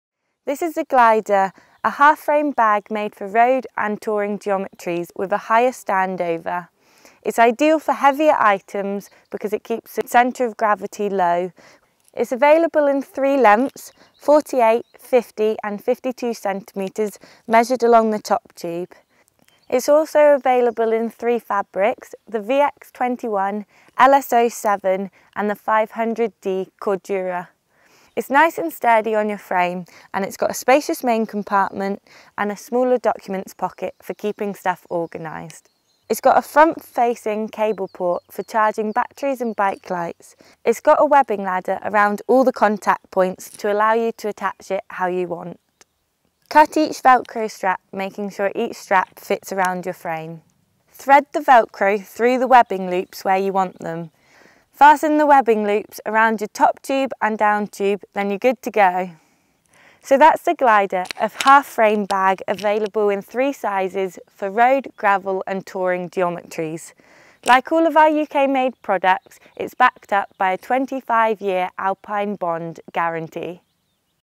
How to fit the Glider bikepacking frame bag
Dusty Italian lanes picking their way round vineyards, quaint yet brutal French cobblestones, skirting round Fjords on smooth tarmac. No matter the adventure, you can count on the Glider.

Glider frame bag sits in your fraont triangle to provide stable storage for your heavier items, keeping your centre of gravity low for minimum effect on handling. The ½ frame design fits a multitude of frames, and at 4 cm wide it’s the perfect addition to your road, cycle touring, or adventure bikepacking setup.

The simple design makes Glider spacious for its size, with a smaller accessory pocket and key hook to keep your wares organised. The front-facing cable port keeps things neat and simple when running wires to your dynamos and charging your light batteries on-the-go during those longer bikepacking adventures.